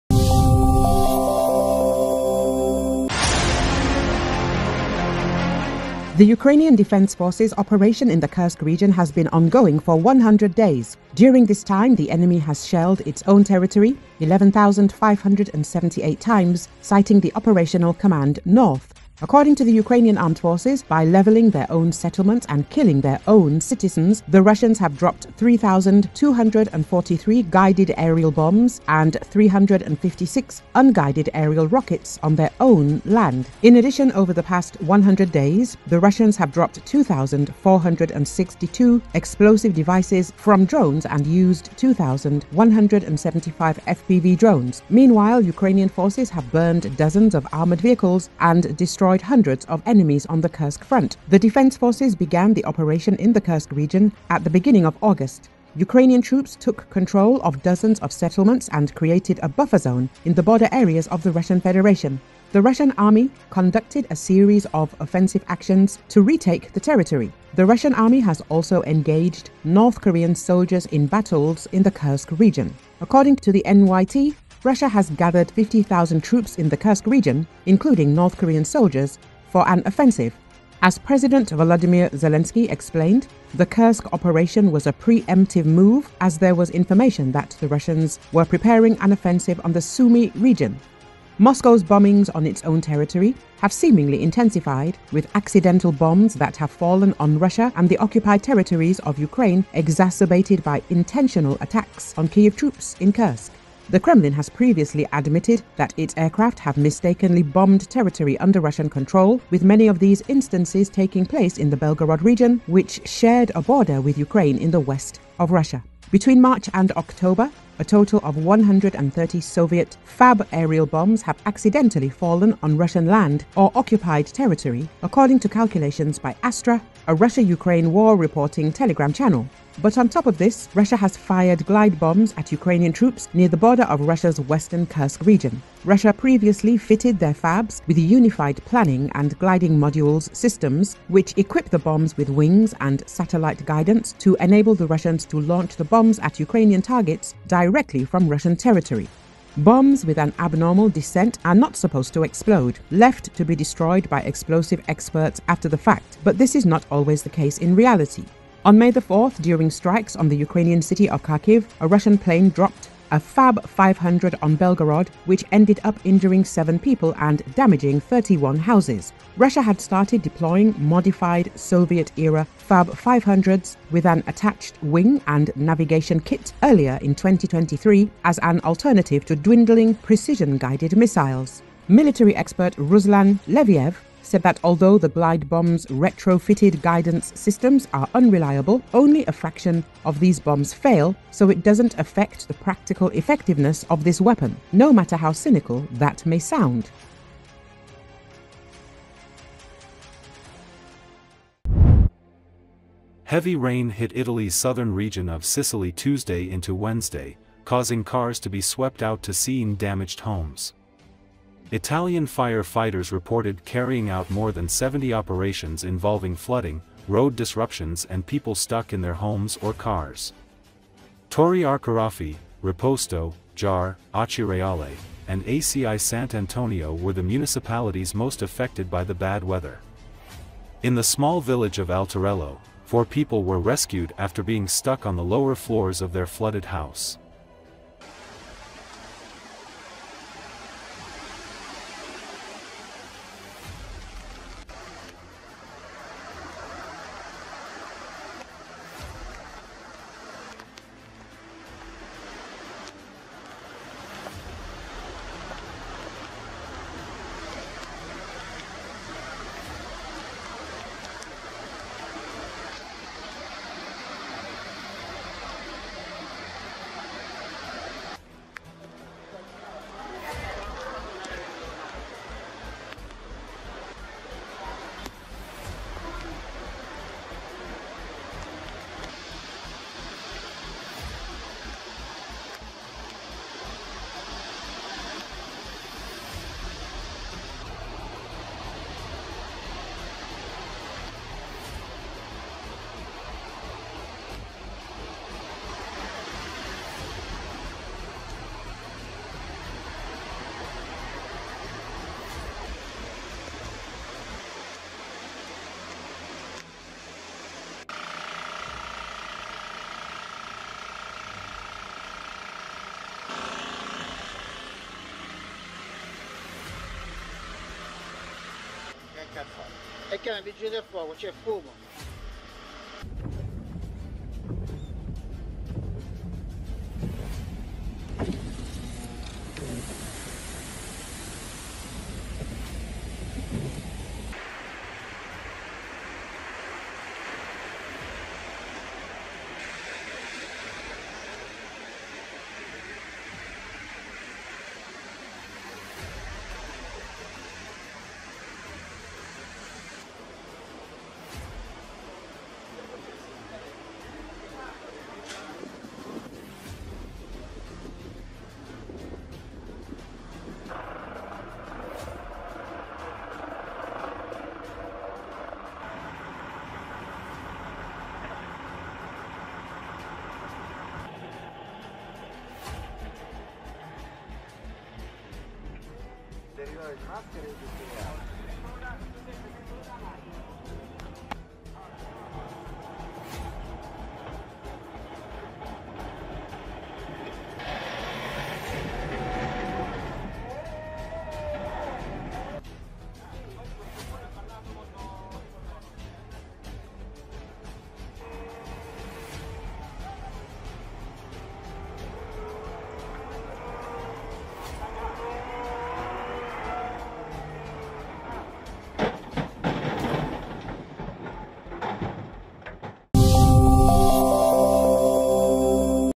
Russia dropped 3.5 thousand bombs on its territory during 100 days of Kursk operation
The Ukrainian Defense Forces' operation in the Kursk region has been ongoing for 100 days. During this time, the enemy has shelled its own territory 11,578 times, citing the Operational Command North.

According to the Ukrainian Armed Forces, by leveling their own settlements and killing their own citizens, the Russians have dropped 3,243 guided aerial bombs and 356 unguided aerial rockets on their own land.

In addition, over the past 100 days, the Russians have dropped 2,462 explosive devices from drones and used 2,175 FPV drones.

Meanwhile, Ukrainian forces have burned dozens of armored vehicles and destroyed hundreds of enemies on the Kursk front.

The Defense Forces began the operation in the Kursk region at the beginning of August. Ukrainian troops took control of dozens of settlements and created a buffer zone in the border areas of the Russian Federation.

The Russian army conducted a series of offensive actions to retake the territory. The Russian army has also engaged North Korean soldiers in battles in the Kursk region. According to the NYT, Russia has gathered 50,000 troops in the Kursk region, including North Korean soldiers, for an offensive.

As President Volodymyr Zelenskyy explained, the Kursk operation was a preemptive move, as there was information that the Russians were preparing an offensive on the Sumy region.

Moscow's bombings on its own territory have seemingly intensified, with accidental bombs that have fallen on Russia and the occupied territories of Ukraine exacerbated by intentional attacks on Kyiv troops in Kursk.

The Kremlin has previously admitted that its aircraft have mistakenly bombed territory under Russian control, with many of these instances taking place in the Belgorod Region, which shares a border with Ukraine in the west of Russia.

Between March and October, a total of 130 Soviet FAB aerial bombs  have accidentally fallen on Russian land or occupied territory, according to calculations by ASTRA, a Russia-Ukraine war reporting Telegram channel. But, on top of this, Russia has fired glide bombs at Ukrainian troops near the border of Russia's western Kursk region.

Russia previously fitted their FABS with Unified Planning and Gliding Modules systems, which equip the bombs with wings and satellite guidance, to enable the Russians to launch the bombs at Ukrainian targets directly from Russian territory.

Bombs with an abnormal descent are not supposed to explode, left to be destroyed by explosive experts after the fact, but this is not always the case in reality.

On May 4, during strikes on the Ukrainian city of Kharkiv, a Russian plane dropped an FAB-500 on Belgorod which ended up injuring seven people and damaging 31 houses.  Russia had started deploying modified Soviet-era FAB-500s with an attached wing and navigation kit earlier in 2023 as an alternative to dwindling precision-guided missiles.

Military expert Ruslan Leviev said that although the glide bombs’ retrofitted guidance systems are unreliable, “only a fraction of these bombs fail, so it doesn’t affect the practical effectiveness of this weapon, no matter how cynical that may sound.”